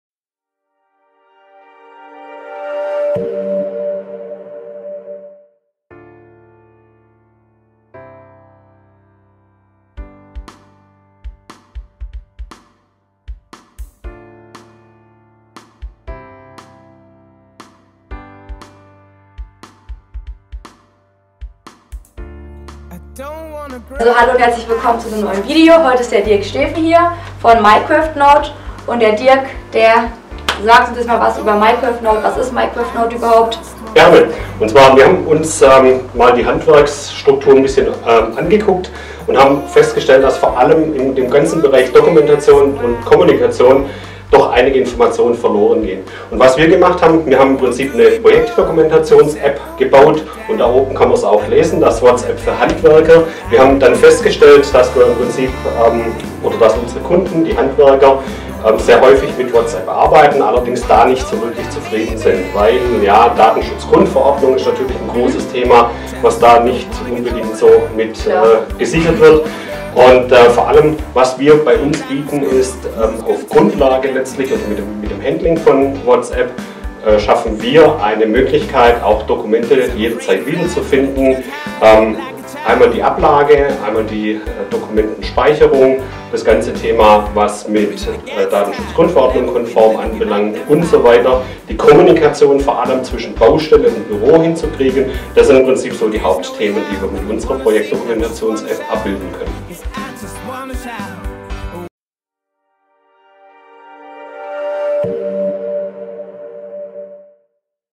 Neuer Partner: MycraftNote
Unser neuer Partner MycraftNote.
In diesem Video stellt sich das Unternehmen MycraftNote vor. Kurz gesagt ist es eine App,dass den Alltag der Handwerker vereinfachen soll.